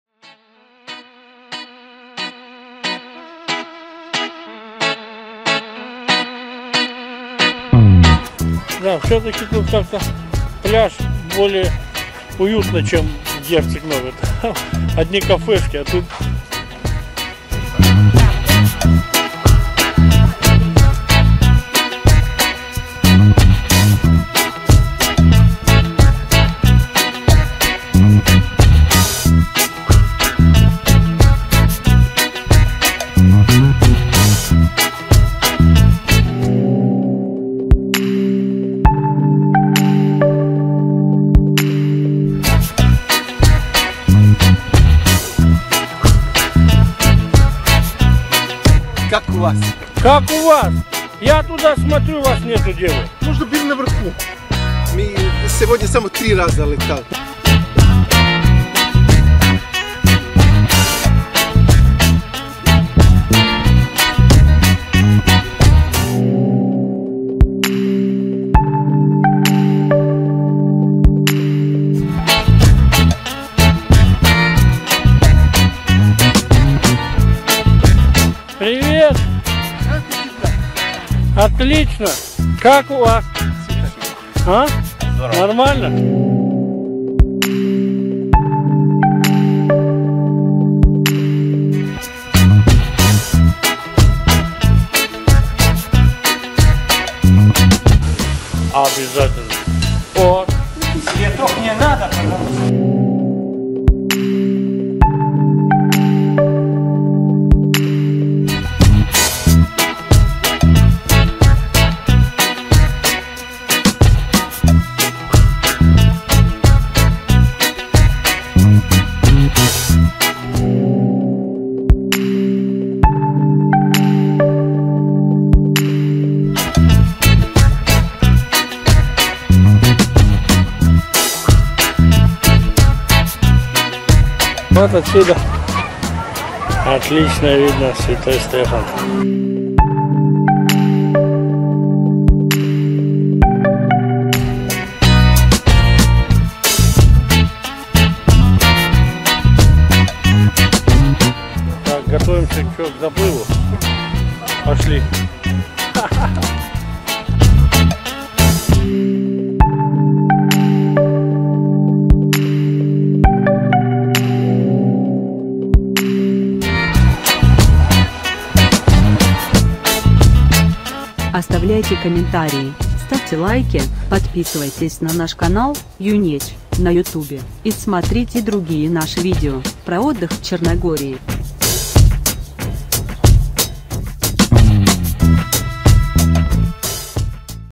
Черногория Набережная в Бечичи (Montenegro The promenade in Becici)
Лучшая набережная в Черногории - набережная в Бечичи! Самые красивые девушки, самые вкусные блюда в кафешках, самые удобные пляжи, самое лучшее море! Самый лучший отдых - отдых в Черногории в Бечичи!

Best waterfront in Montenegro - bečići promenade! The most beautiful girls, the most delicious meals in the cafes, the most comfortable beaches, the best sea! The best holiday - a holiday in Montenegro in Becici!